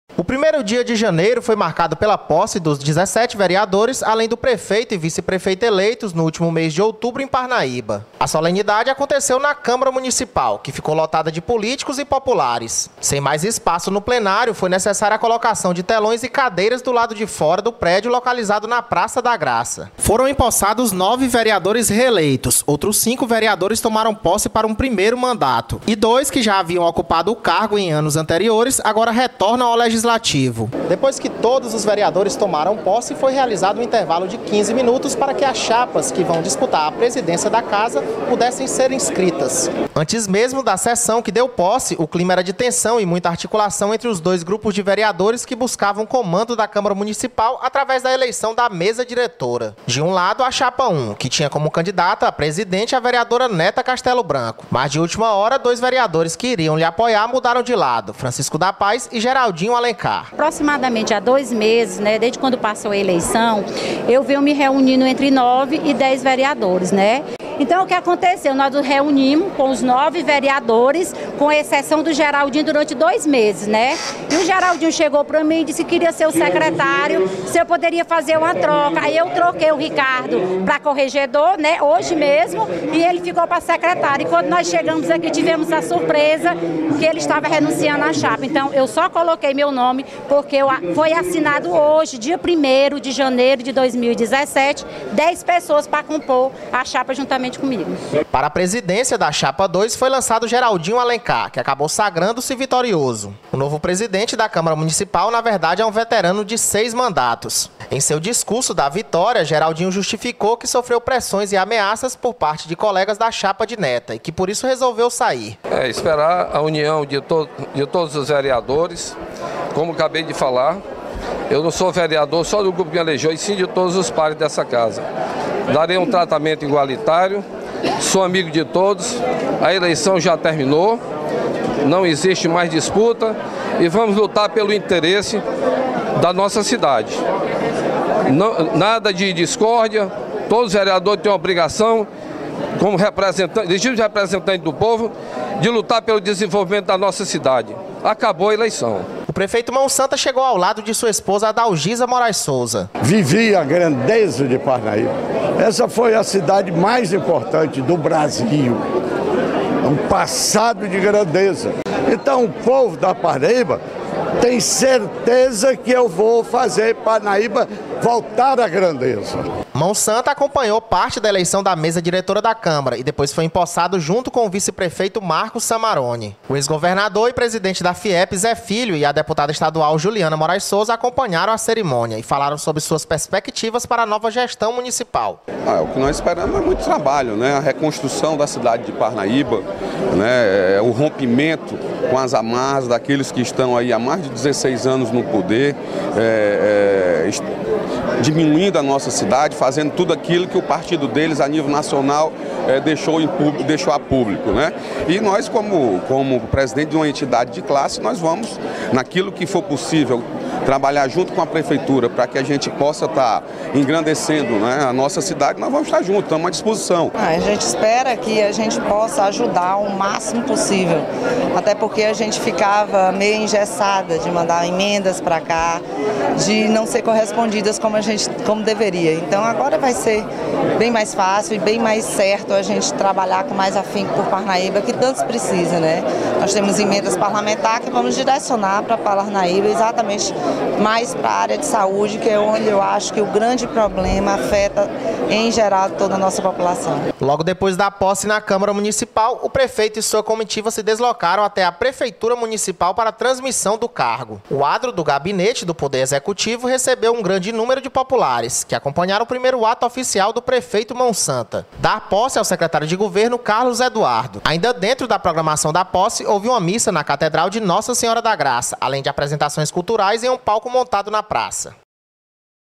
Vereadores tomam posse e elegem mesa diretora da Câmara Municipal
A Câmara Municipal de Parnaíba realizou neste domingo (01) a solenidade de posse dos 17 vereadores eleitos para a legislatura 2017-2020.